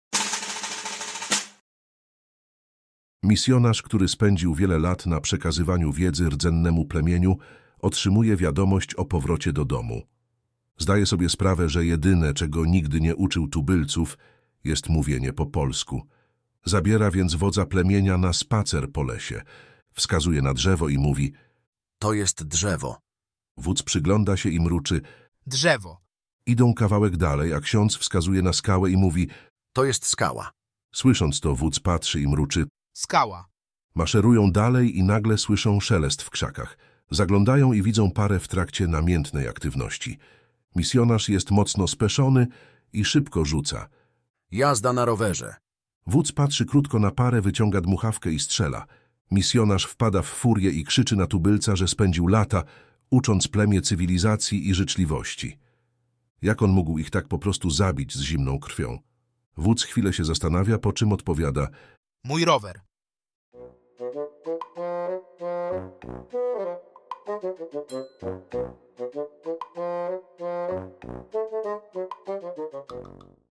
Ksiądz na misji uczy wodza plemienia języka polskiego 😁 Dobry kawał / Dowcip 😁
Misjonarz, który spędził wiele lat na przekazywaniu wiedzy rdzennemu plemieniu, otrzymuje wiadomość o powrocie do domu.
Zdaje sobie sprawę, że jedyne, czego nigdy nie uczył tubylców, jest mówienie po polsku.

Zapraszam do słuchania/czytania.

Dziękuję za obejrzenie, miłego dnia!

@KawalKanalu - regularnie nowe wrzuty.

Dowcipy/Kawały/Żarty/Suchary